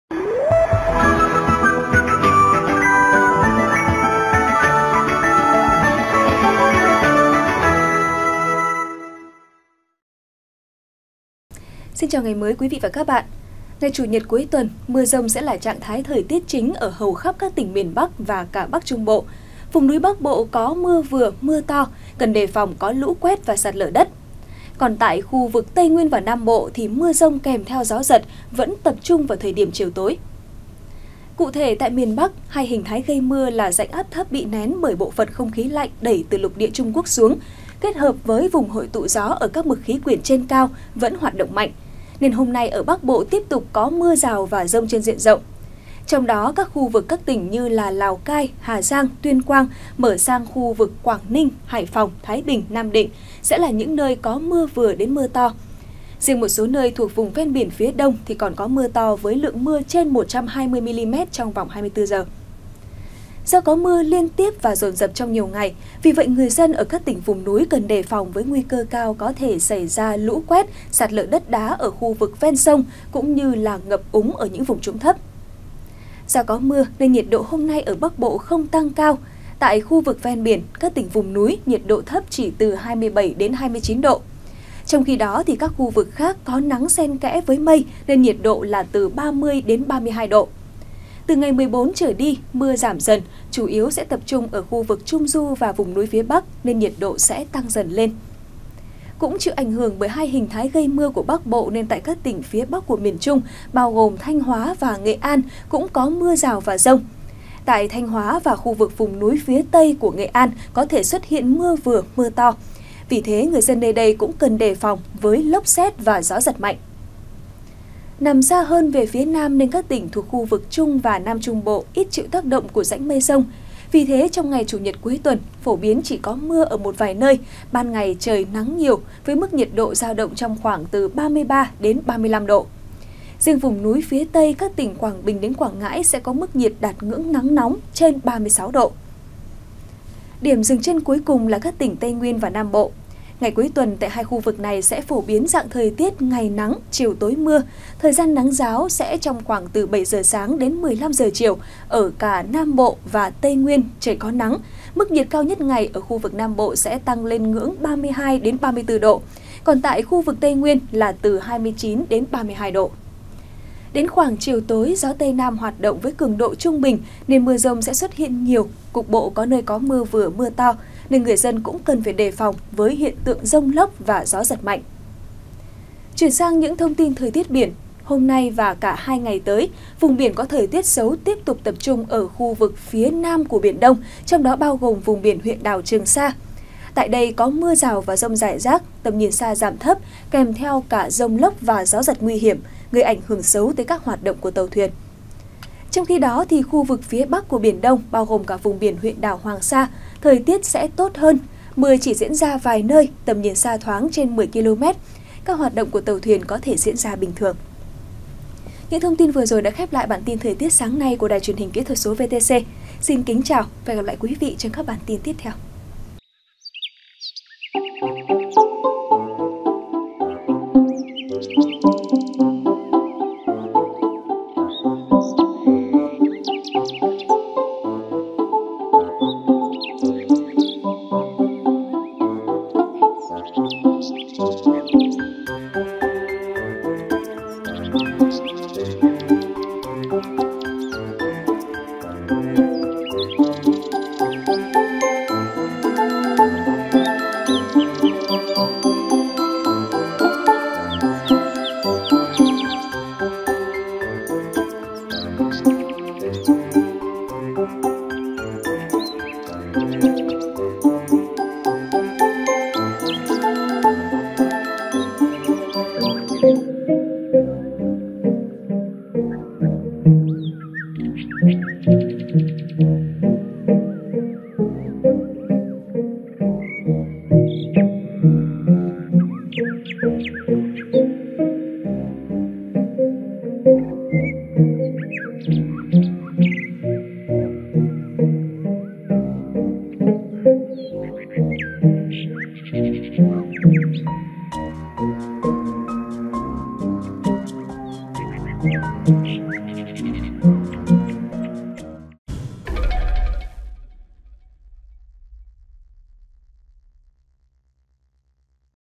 Thời tiết hôm nay 13/09/2020: Mưa dông khắp Bắc Bộ | VTC14
VTC14 | Ngày Chủ Nhật cuối tuần, mưa dông sẽ là trạng thái thời tiết chính ở hầu khắp các tỉnh miền Bắc và Bắc Trung Bộ, vùng núi Bắc bộ có mưa vừa, mưa to, đề phòng lũ quét và sạt lở đất, còn tại Tây Nguyên và Nam Bộ mưa dông kèm theo gió giật vẫn tập trung về chiều tối